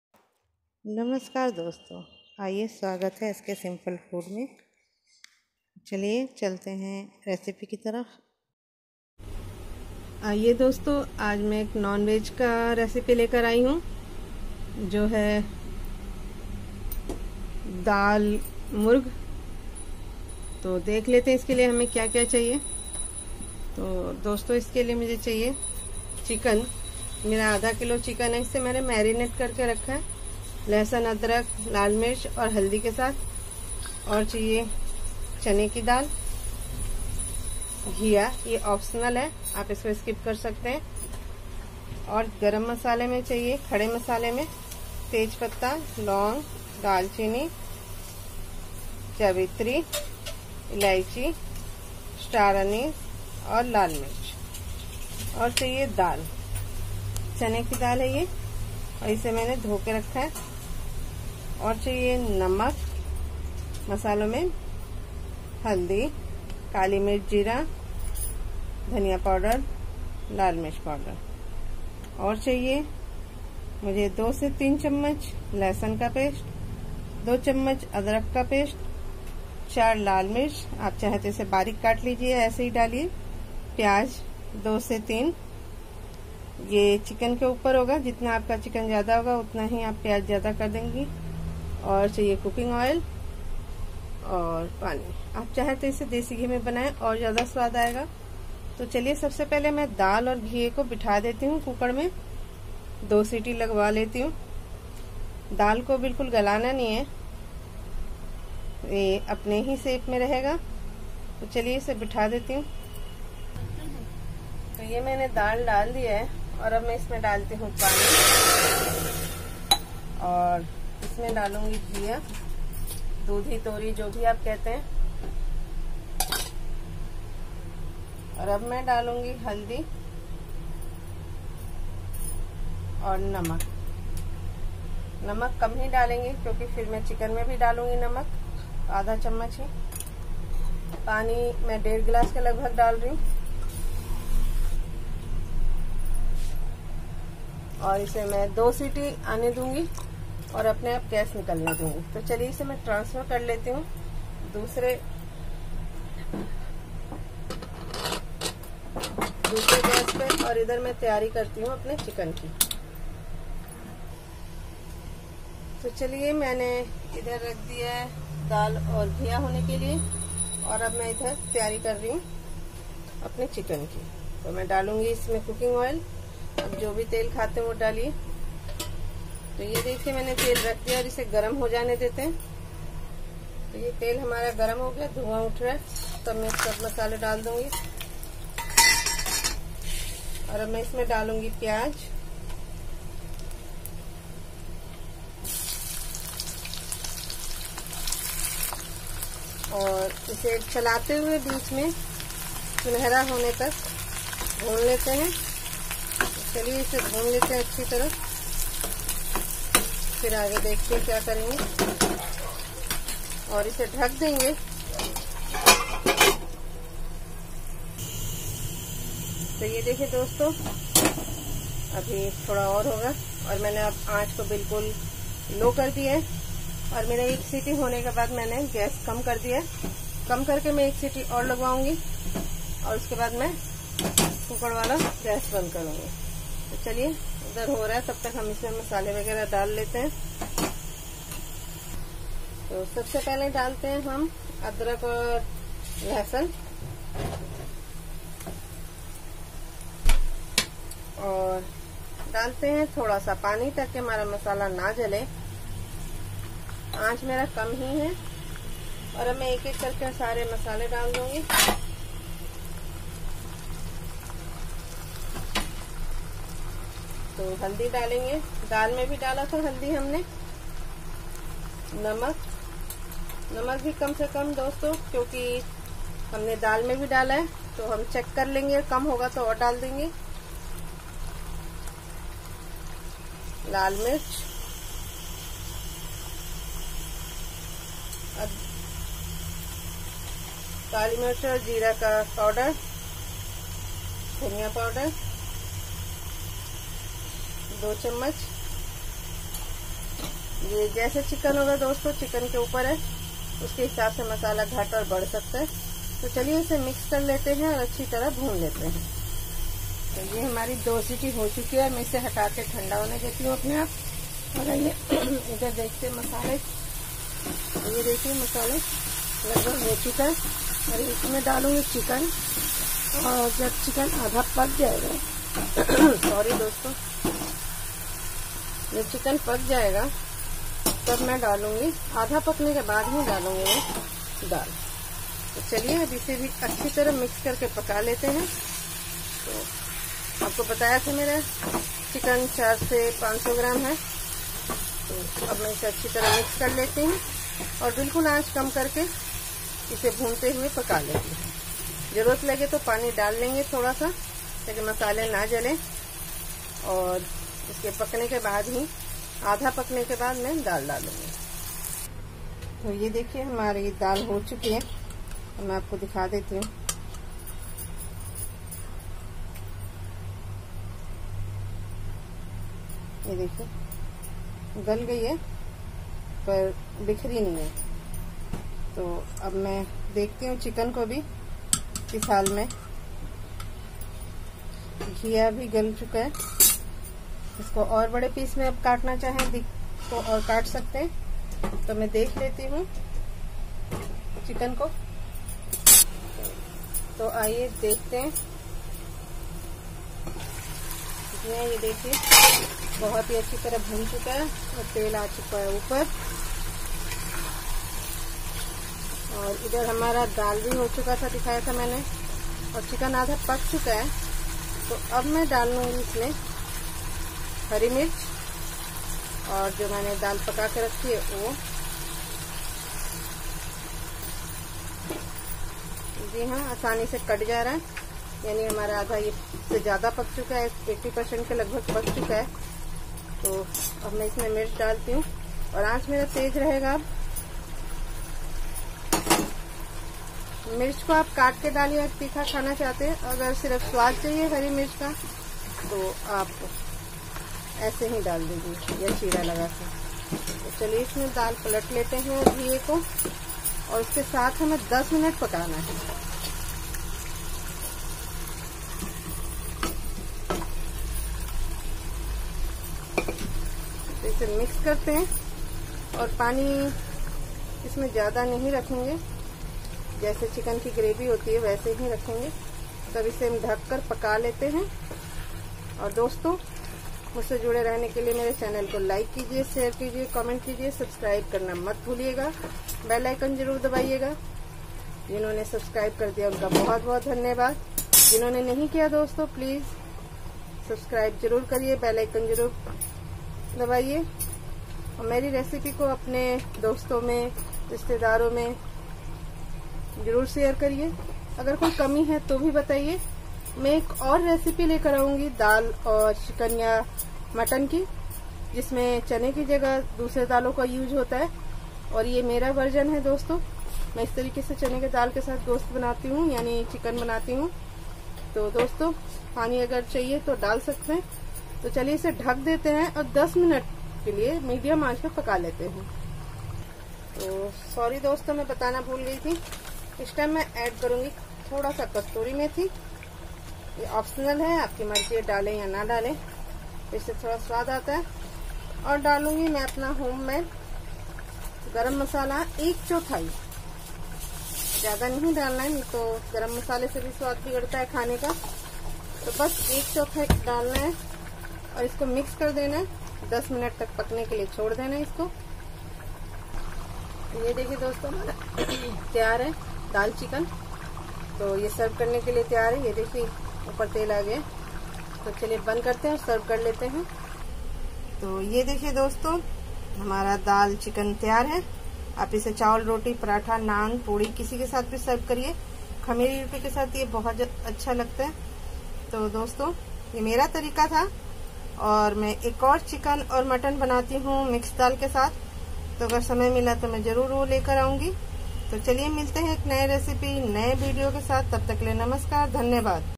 Dal Murg ll Chana Dal Chicken ll Chicken with Lentils
Dal Murg is very easy and quick recipe.. It cooked with Chicken, Lentils and Bottle gourd.. there are many styles to make this recipe..we eat with roti ,rice, naan, paratha and rumali roti..